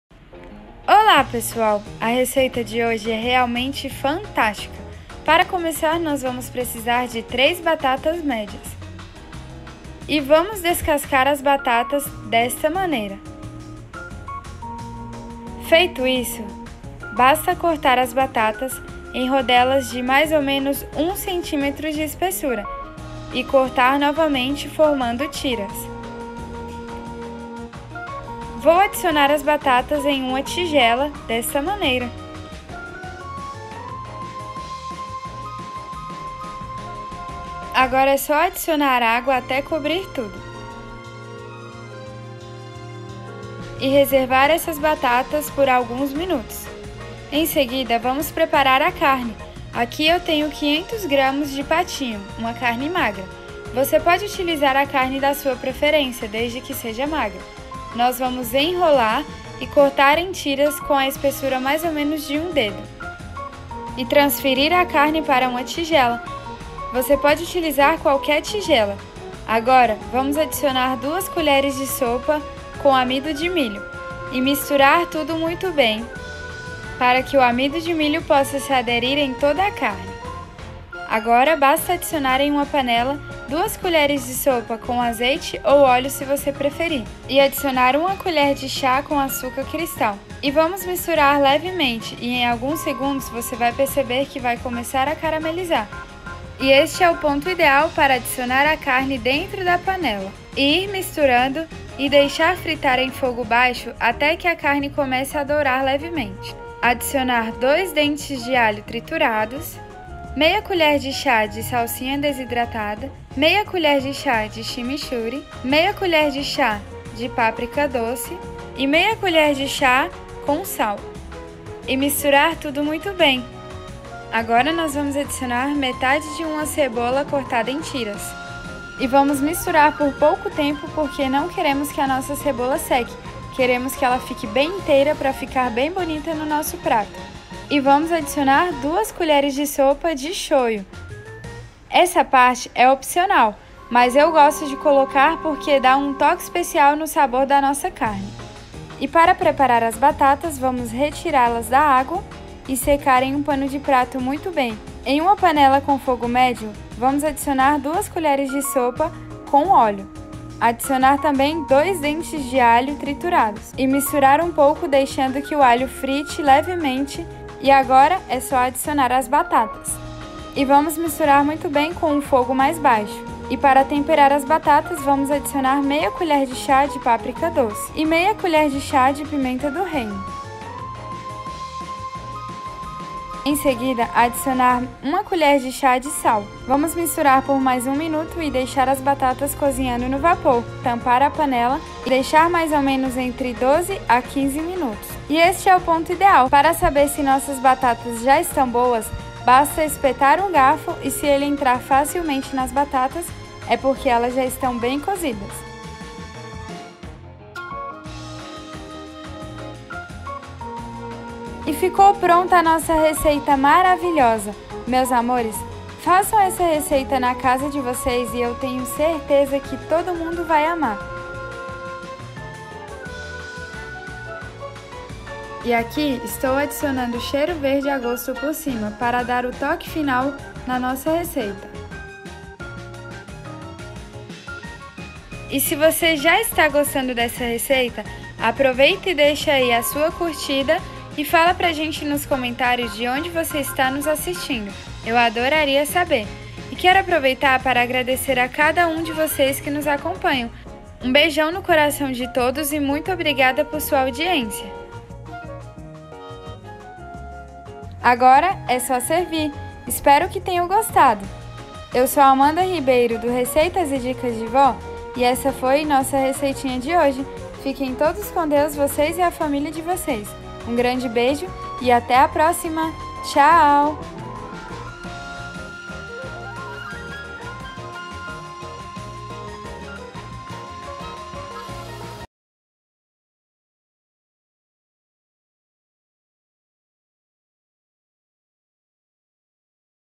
RECEITA DE CARNE COM BATATA FÁCIL E DELICIOSA
Ingredientes:
E batatas
500gr de patinho
2 colheres de sopa de amido de milho
Oleo ou azeite
1 colher de açúcar
4 dentes de alho
1/2 colher de chá de salsinha desidratada
 chimichurri
 páprica
sal
1/2 cebola
2 colheres de sopa de molho shoyu
Pimenta do reino
Cheiro verde

Nosso Instagram